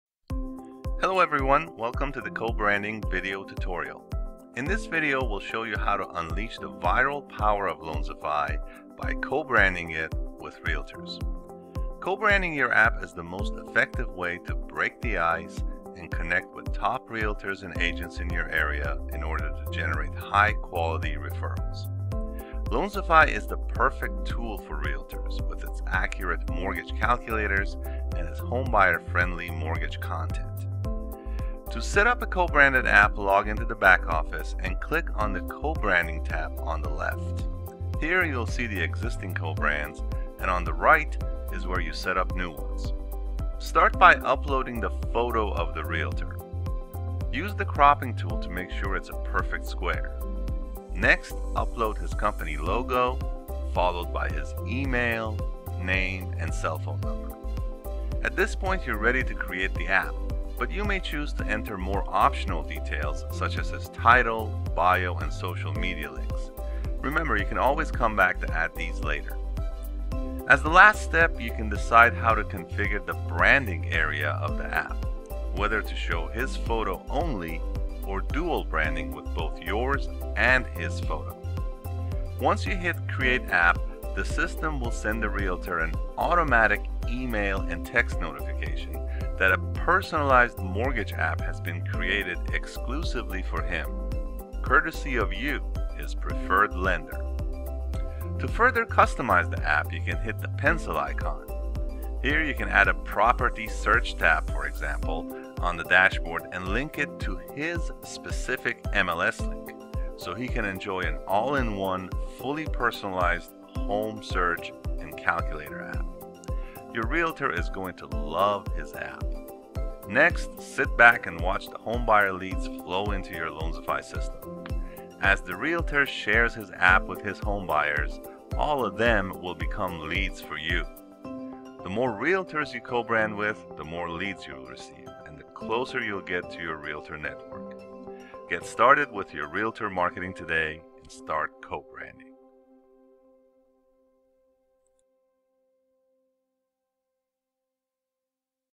Loanzify - Co-Branding with Realtors and Agents
Welcome to the Co-Branding video tutorial.

In this video we will show you how to unleash the viral power of Loanzify by co-branding it with realtors.

Co-branding your app is the most effective way to break the ice and connect with top realtors and agents in your area in order to generate high qualify referrals.

Loanzify is the perfect tool for Realtors with its accurate mortgage calculators and its home buyer friendly mortgage content.

To set up a co-branded app, login to the back-office and click on the co-branding tab on the left. Here you will see the existing co-brands, and on the right is where you set up new ones.

Start by uploading the photo of the realtor. Use the cropping tool to make sure it is a perfect square. Next upload his company logo followed by his email, name, and cell phone number. At this point, you are ready to create the app, but you may choose to enter more optional details such as his title, bio, and social media links. Remember you can always come back to add these later.

As the last step you can decide how to configure the branding area of the app: whether to show his photo only, or dual branding with both yours and his photo.

Once you hit “Create app”, the system will send the realtor an automatic email and text notification that a personalized mortgage app has been created exclusively for him courtesy of you, his preferred Lender.

To further customize the app, you can hit the “pencil icon”. You can add a property search tab on the dashboard and link it to his specific MLS link so he can enjoy an all-in-one fully-personalized home search and calculator app.

Your realtor is going to LOVE his app.

Next sit back and watch home buyer leads flow in to your Loanzify system. As the realtor shares his app with his home buyers, all of them will become leads for you. And the more realtors you co-brand with, the more leads your will receive and the closer you will get to your realtor network. Get started with your realtor marketing today and start co-branding!